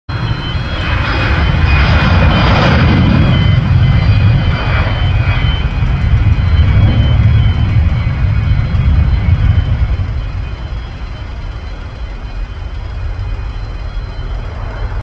easyjet drying the runway on departure
Easyjet Airbus A319 drying the runway after a heavy shower on route to Scotland. Over the past few week Easyjet have opened up a number of routes at BHX including CDG, LYS, and plan to open a base at bhx next summer